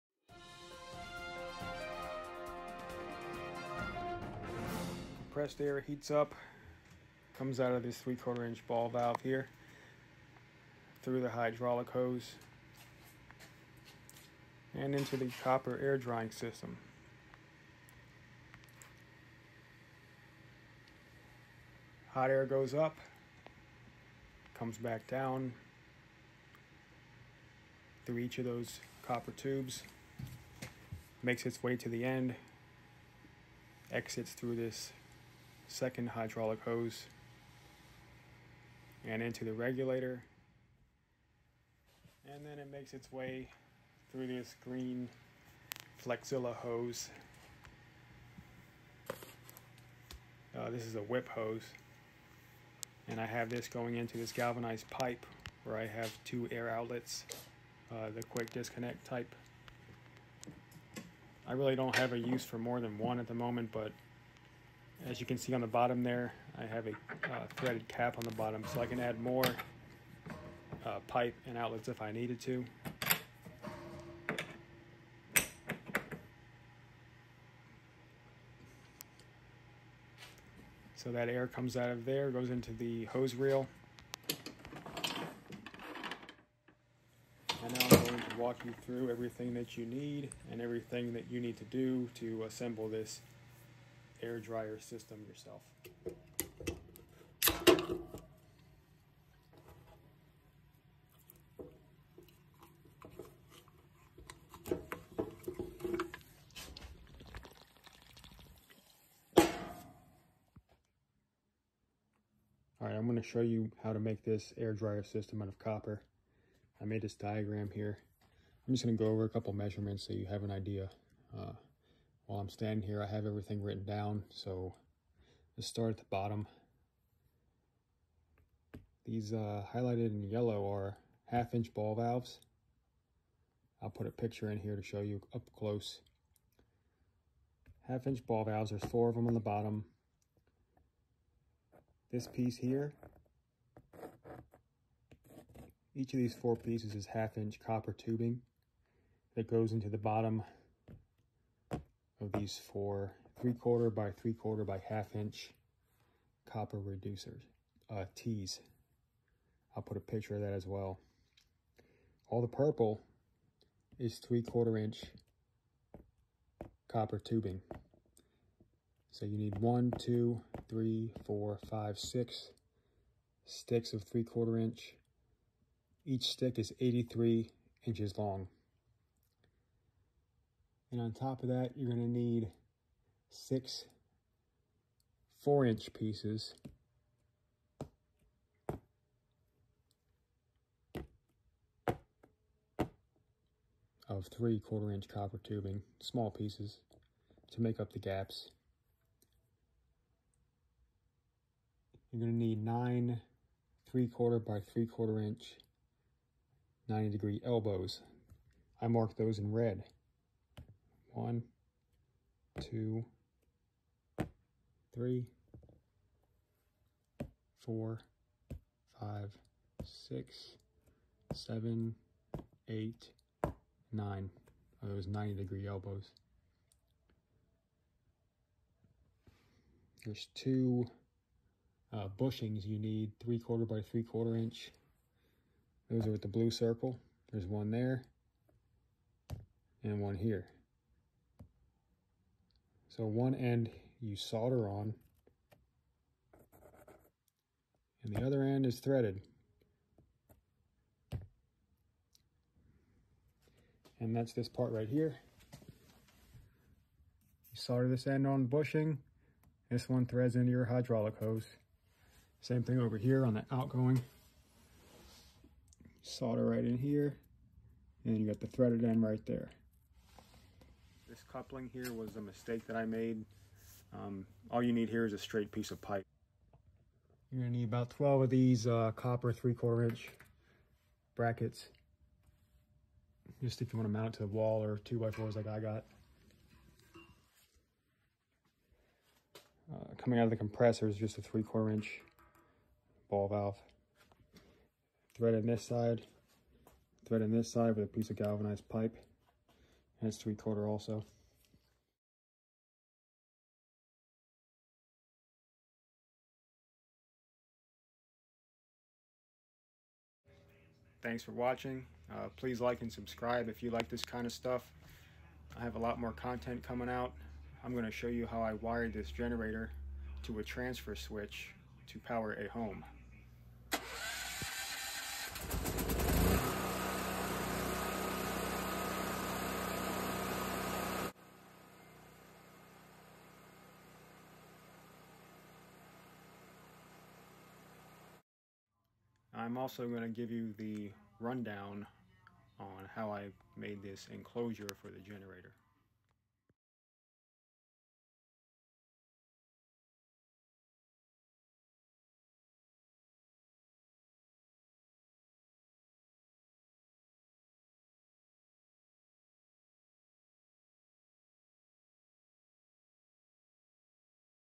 Compressed Air Dryer System - How To Build
In this video, I walk you through everything you need to make your own air dryer system for your air compressor. When compressed air is generated, it is very warm and full of moisture. This can cause many problems when spraying paint, sandblasting, using pneumatic tools, or even just adding air to your tires. COOL, DRY air is what you want coming out of your hose. This copper air dryer system will do the trick.